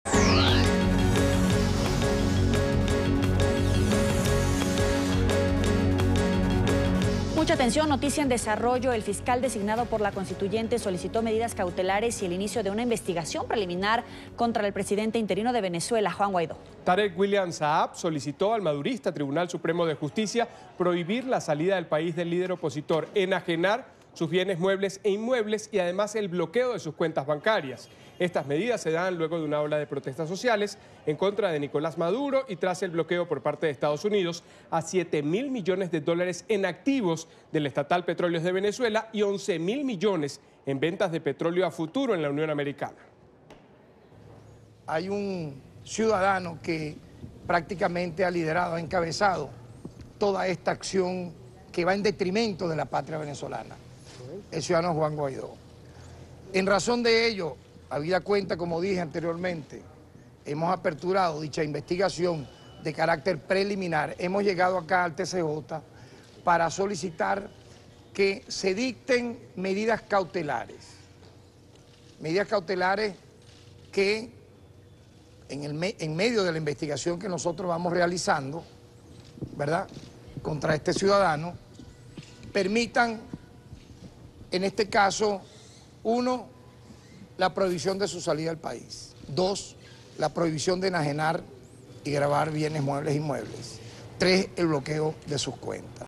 Fiscal general de Venezuela pide dictar medidas cautelares contra Juan Guaidó
El fiscal general de Venezuela, Tarek William Saab, pidió este martes al Tribunal Supremo de Justicia que prohíba la salida del país caribeño y congele las cuentas del presidente interino venezolano, Juan Guaidó.